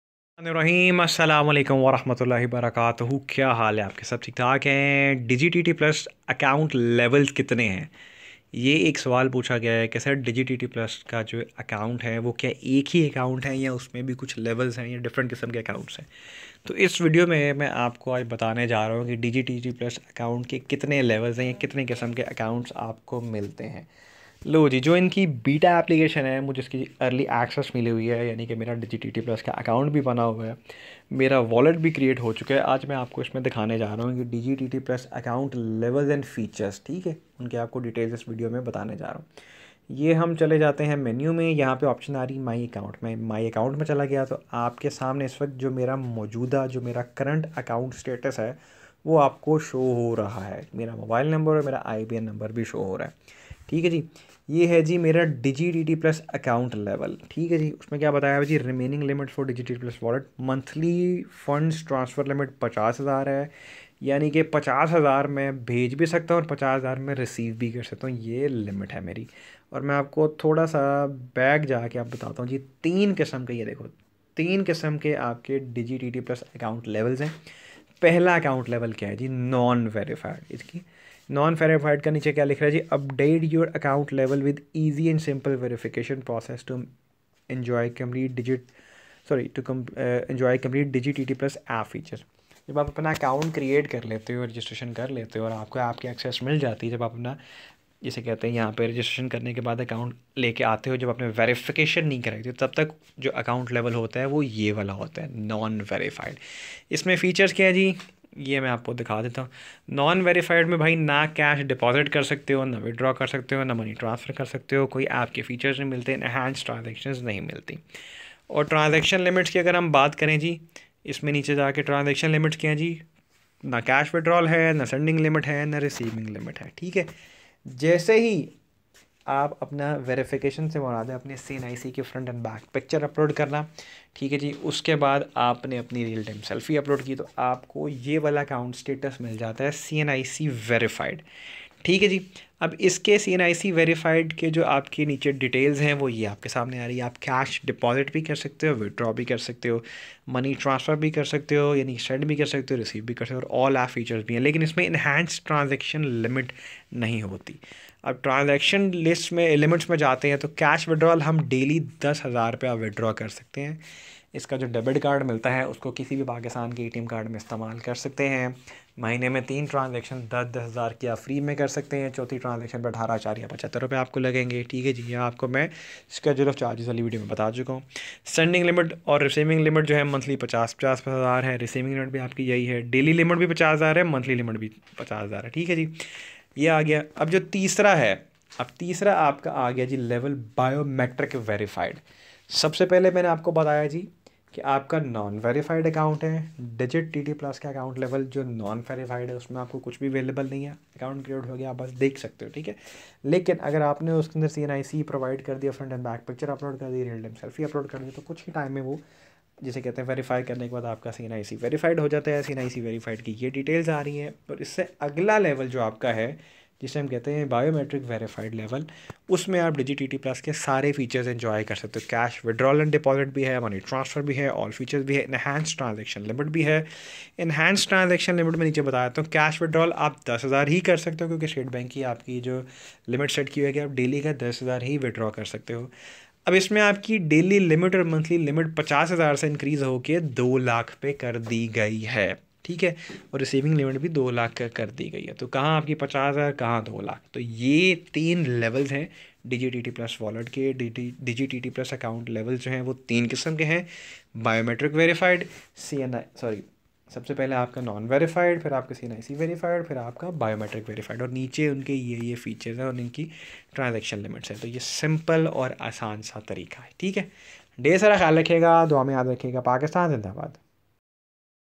Digitt+ Account Levels, Features & Transaction Limits | 3 Different Levels of Digitt+ Wallet Account
Digitt+ Account Levels, Features and Transaction Limits | Three (3) Different Levels of Digitt+ Wallet Account